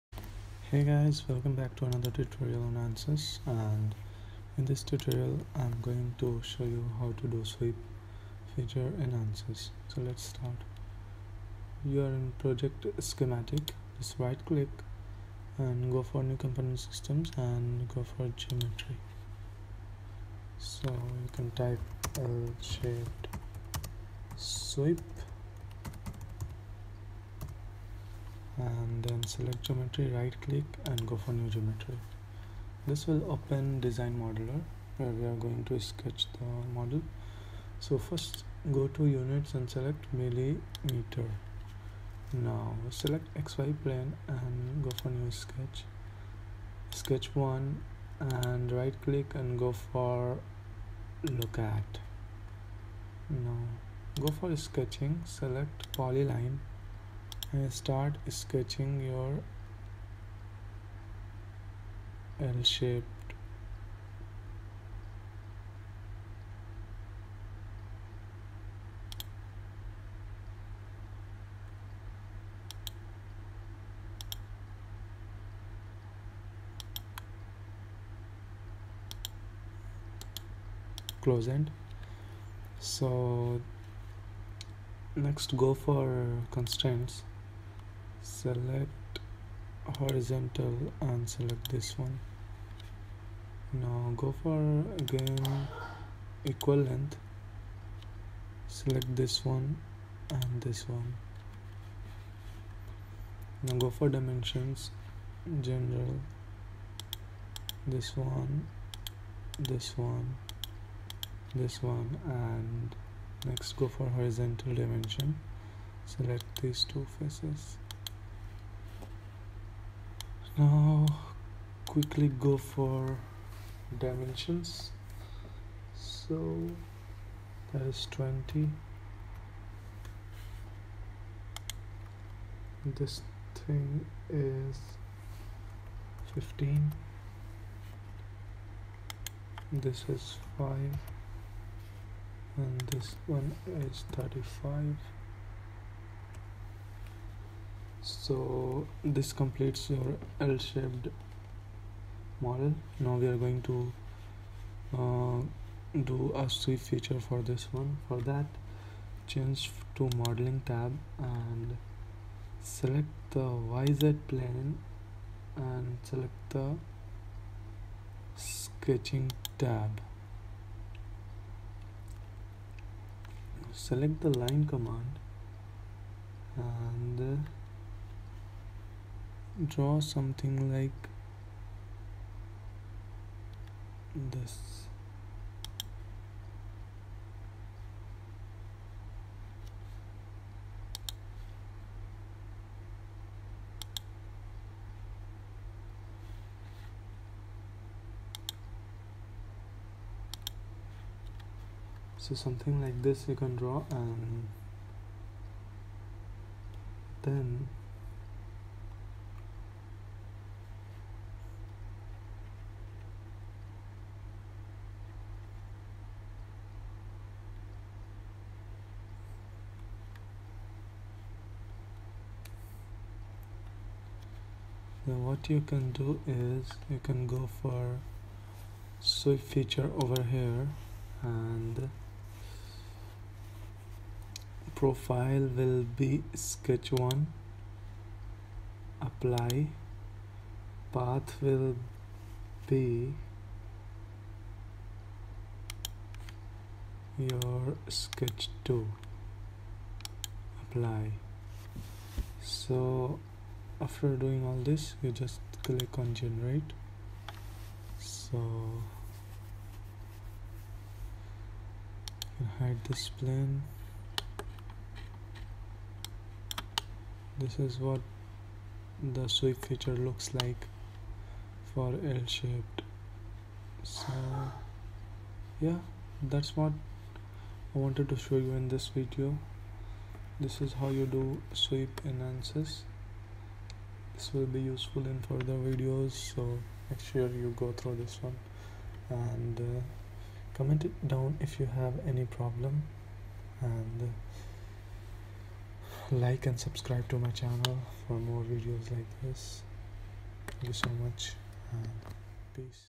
ANSYS - Sweep
this tutorial shows how to use sweep feature in ansys workbench.
comment if u have any doubts